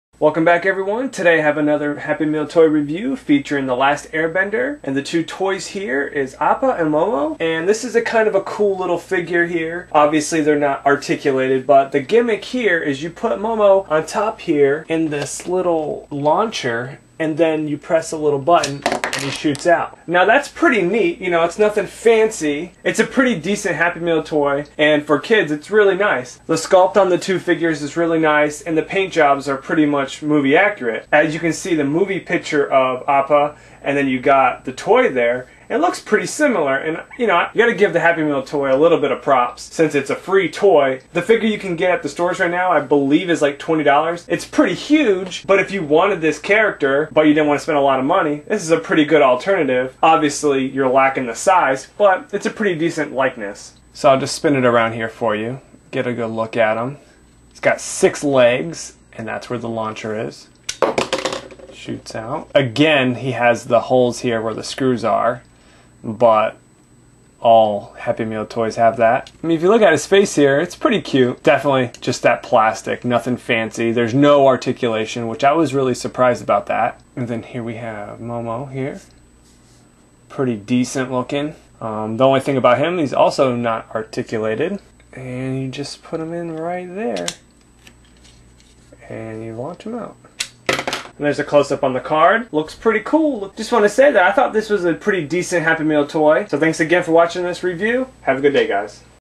Appa & Momo - The Last Airbender Happy Meal Toy Review
This Happy Meal toy is from the Latest film The Last Airbender with Appa and Momo it also says its the #2 figure, but Im thinking you can get girl toys or boy toy?